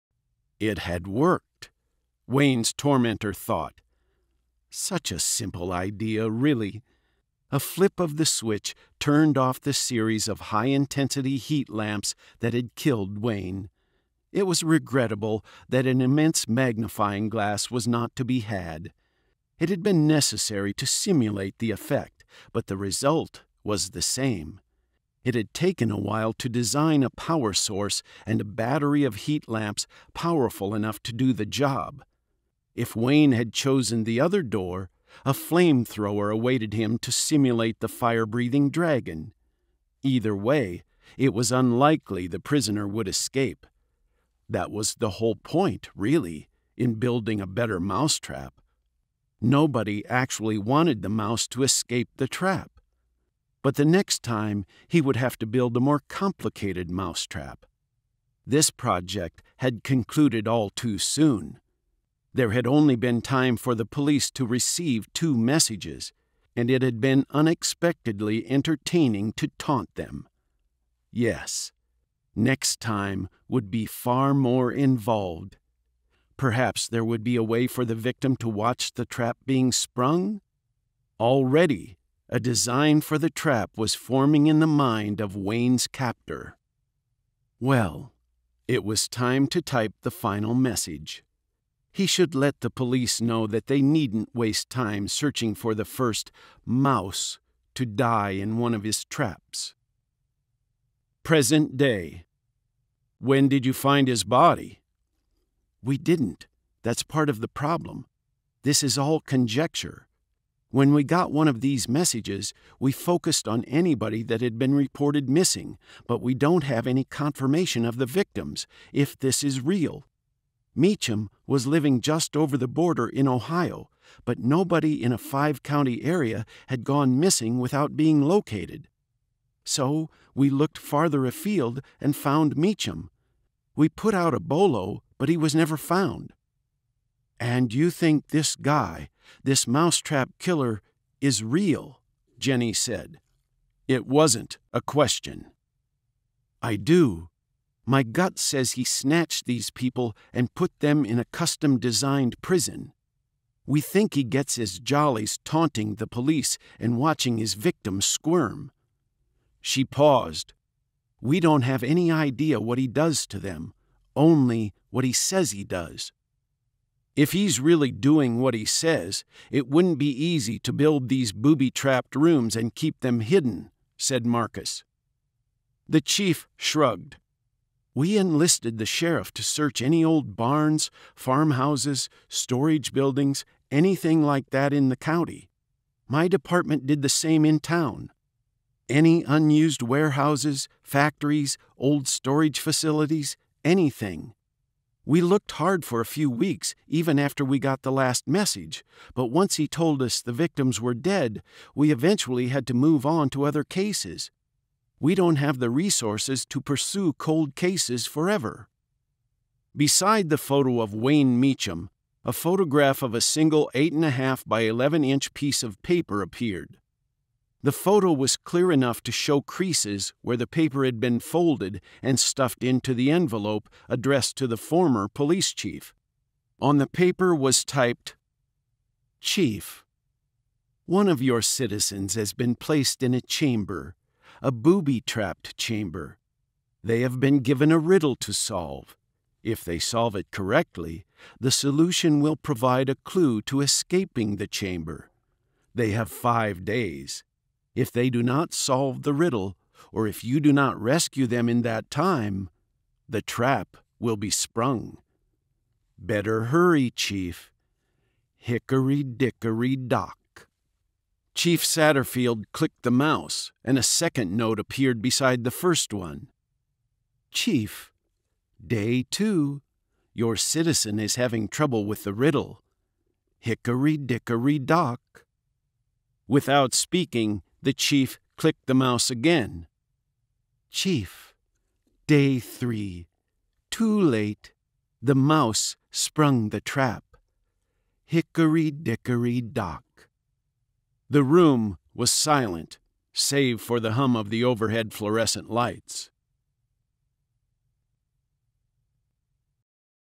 The Mousetrap Killer: Marcus Lear Mysteries, Book 3 [Audiobook Sample]
Full audiobook available on Audible and iTunes.

Narrated by Marlin May

Publisher's Summary
Former police detective Sergeant Marcus Lear’s career was ended by PTSD, the result of a gunfight with a 16-year-old burglary suspect. He has moved on by founding a company that is developing software to help law enforcement agencies solve complex criminal cases.

Now, the serial murderer known as The Mousetrap Killer claims to have struck again. Three times in the past, he announced to the police chief of Maryvale, Indiana, that an innocent victim had been taken and imprisoned in an elaborate chamber, cleverly booby-trapped and set to kill if the victim could not solve a riddle in time. The riddle would provide a clue to escaping the trap. Every day, the Killer would taunt the police until the trap was sprung and the victim died. But no victim’s body had ever been recovered, and the killer was never identified.

Now, the killer has sent another message announcing a new trap, and a new victim. Who has been dropped into that terrible prison this time? Where are they being held? And, who is the Mousetrap Killer? Desperate, the Maryvale police call on Marcus and his team to use their new and untested software application to help find the killer and the latest victim. Can the Lear team and Kat Montague, young Asian goth detective and Lear’s newest field agent, work with the local police and FBI to find the victim before time runs out?

The task force swings into action while awaiting the next message from the killer, taunting the police and perhaps providing a clue to saving the victim. But why has the killer gone silent?! While pondering this new mystery, a frightening possibility is uncovered regarding the latest victim that hits very close to home. Marcus and his people must find the trap, and the victim, before it’s too late!